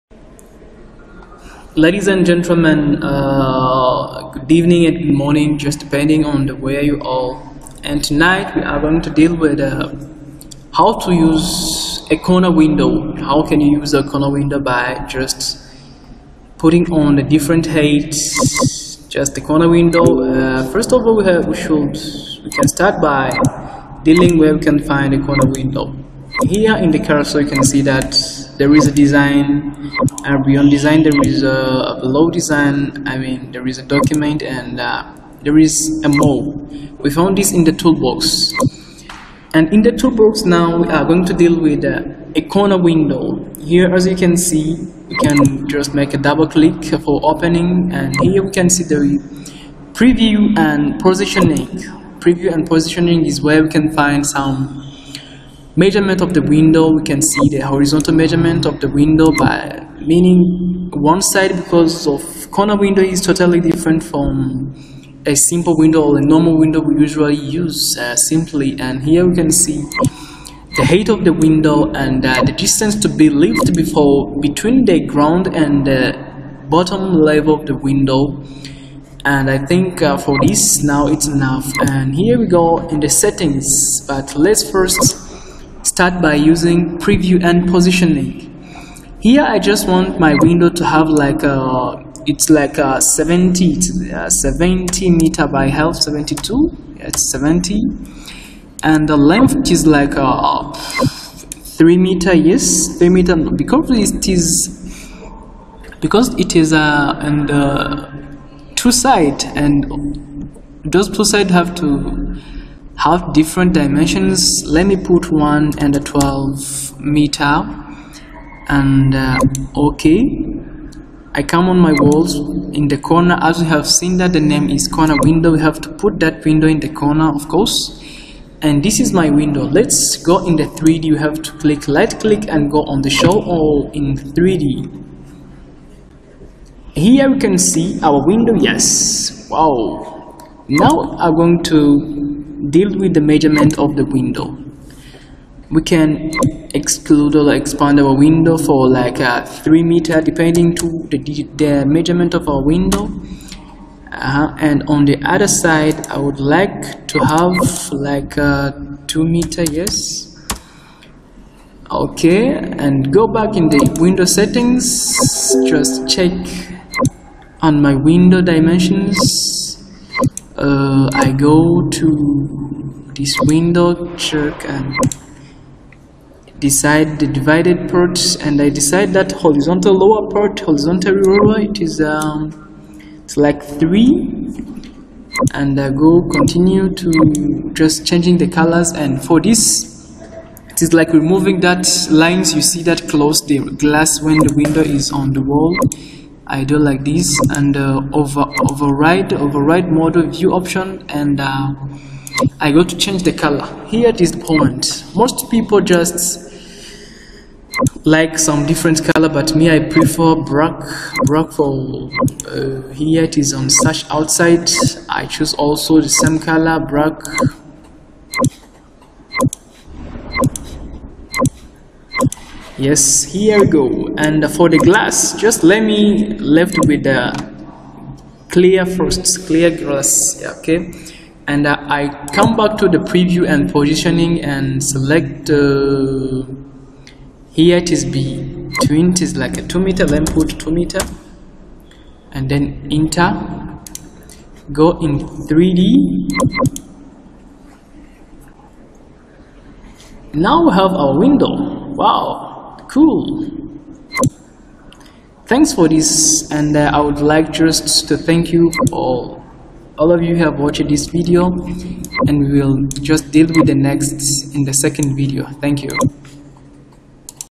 New Archicad Tutorial @ Corner window design
This video is all about how can use a corner window.